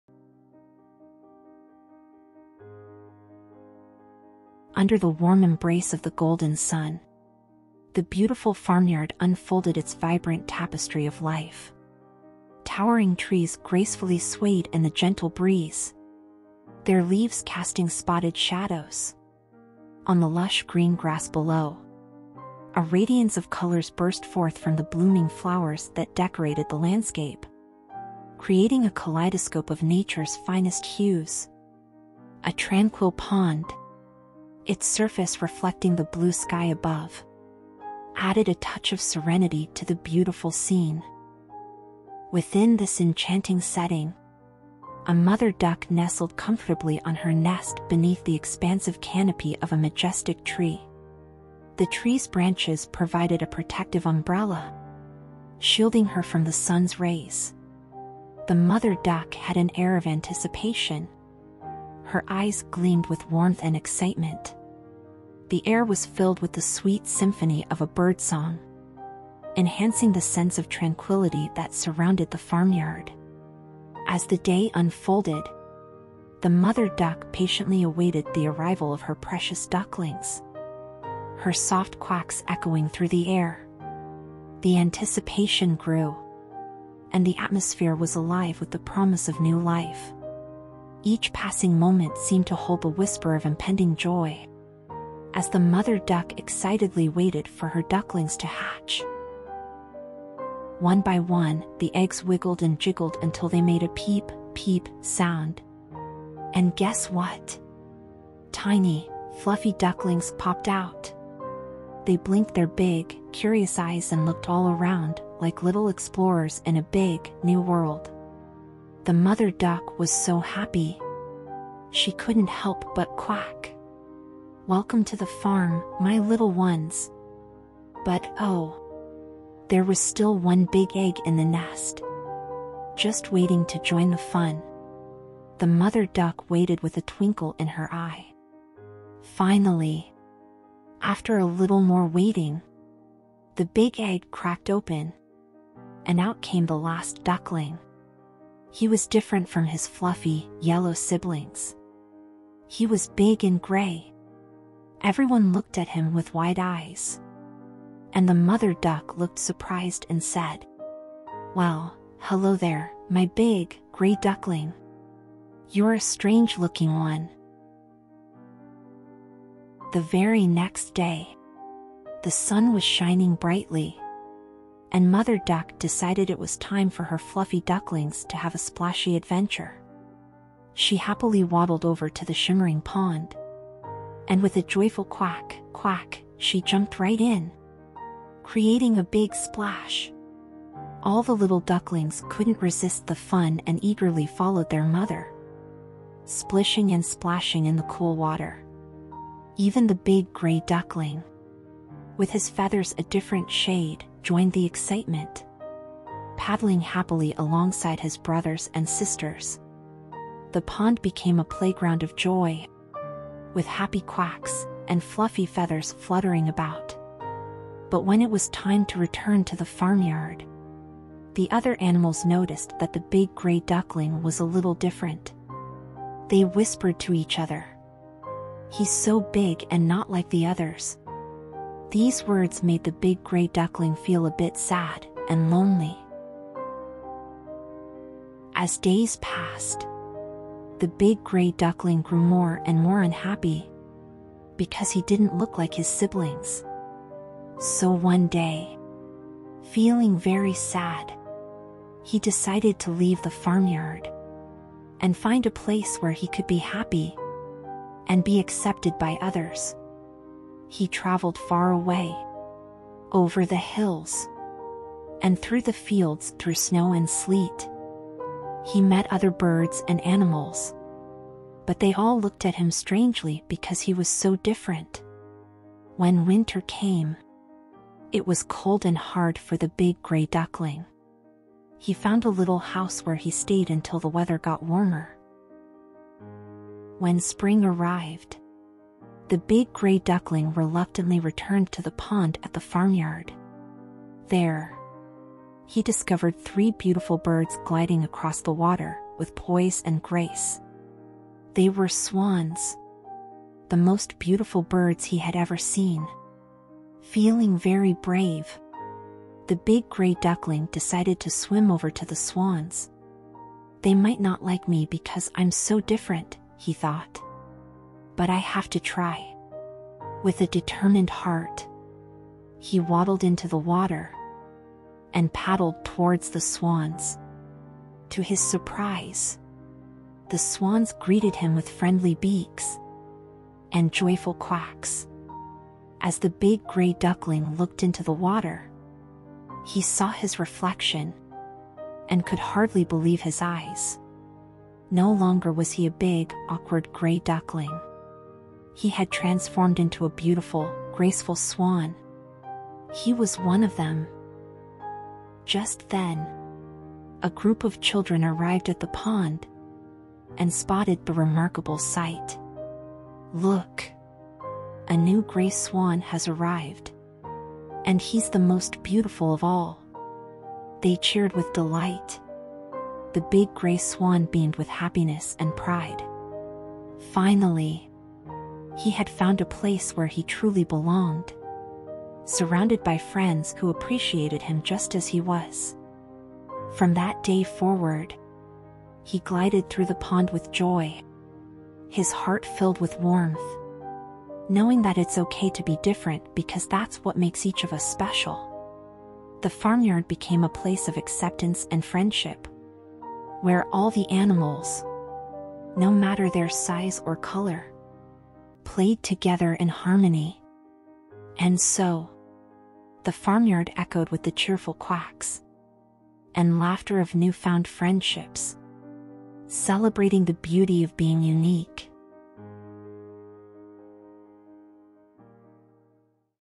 The Ugly Duckling | Bedtime Fairytale Stories for Children | Sleep Meditation for Kids and Babies
"The Ugly Duckling" is a relaxing and soothing bedtime sleep meditation story designed for children. Join us on a magical bedtime sleep journey that is perfect for babies, toddlers, and big kids.
Join The Ugly Duckling on a heartwarming adventure as he embarks on a quest of discovery, acceptance, and the joy of finding one's place in the world. A delightful tale that will warm your heart and remind you that sometimes it's okay to be different because that's what makes each of us special.

🎥 Dive into our growing collection of bedtime stories, meditation tales, and animated wonders, all designed to create a serene and joyous bedtime routine. 🌜🌙✨